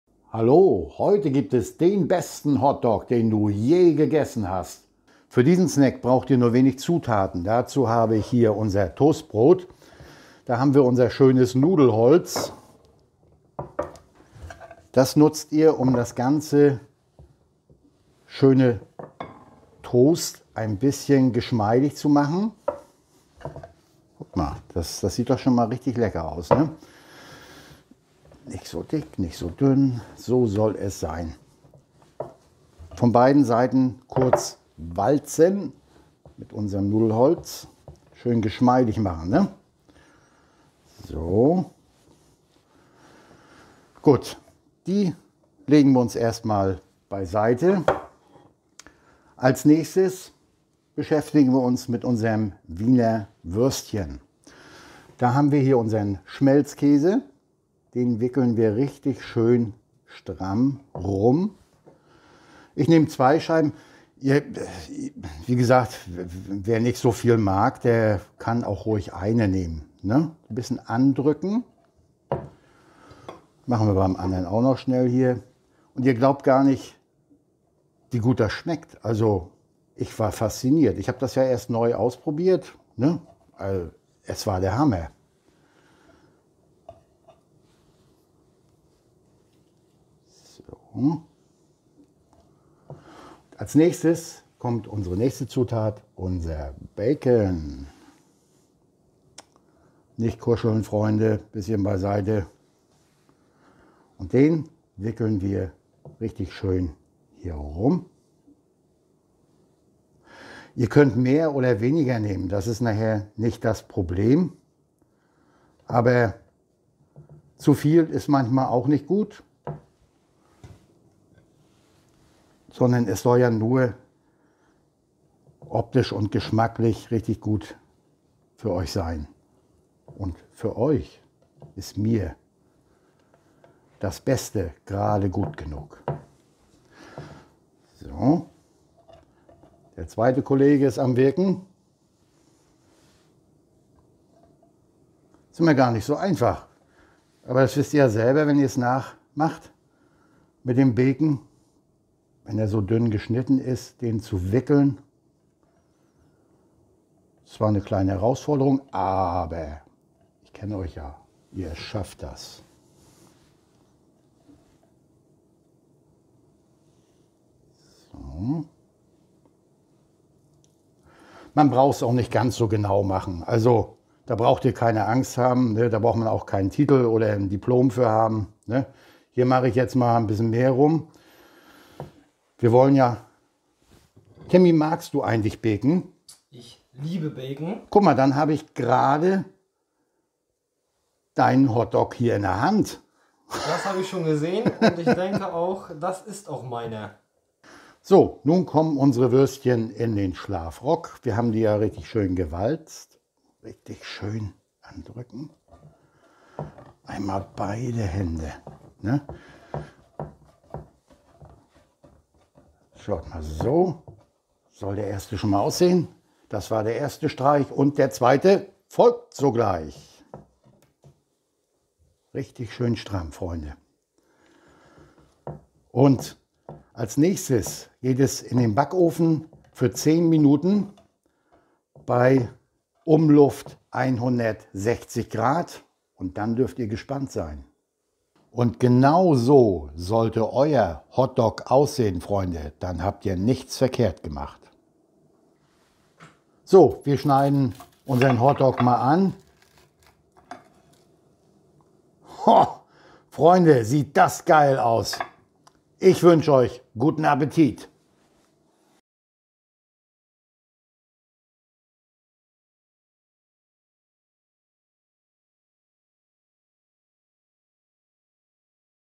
Hotdog Rezept mal anders - Unglaublich Lecker !
Der Leckerste Hotdog den wir jemals gegessen haben!
Heute zeigen wir euch ein Hotdog Rezept was mal ein bisschen anders ist,
aber trotzdem super einfach und mega lecker ist !

Was du für dieses Hotdog Rezept benötigst?

2 Scheiben Toastbrot
2 Scheiben Wiener Würstchen
4 Scheiben Schmelzkäse
4-6 Scheiben Bacon

Als aller erstes walzt du dein Toastbrot mit einem Nudelholz oder ähnlichem platt. Danach wickelst du deine Wiener Würstchen mit Schmelzkäse und Bacon ein. nun rollst Du deine Wieder Würstchen in das Toast ein und gibst diese bei 160° Grad Umluft für ca. 10 Minuten in den Backofen.

Fertig ist dein Hotdog mal anders.
Lass es Dir schmecken!